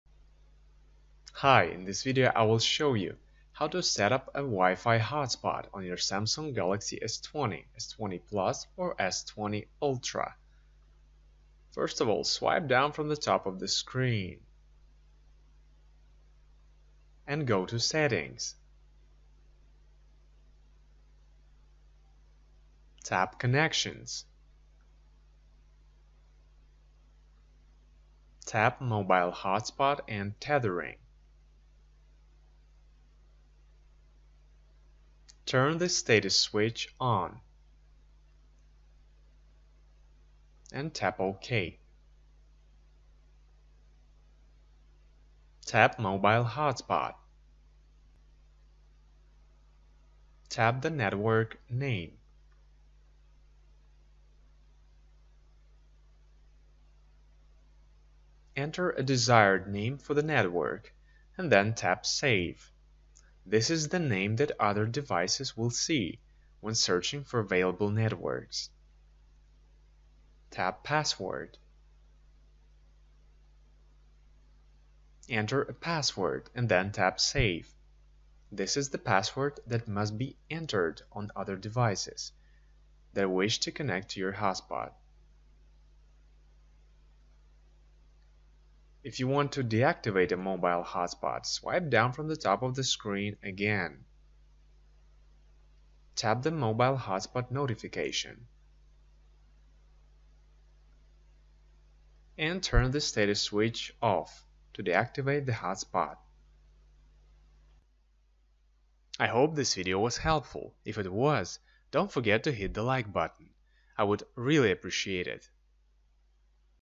Wi-Fi hotspot on Samsung Galaxy S20, S20 Plus, or S20 Ultra, How to
In this video, I explain how to set up a Wi-Fi hotspot on your Samsung Galaxy S20, S20 Plus, or S20 Ultra.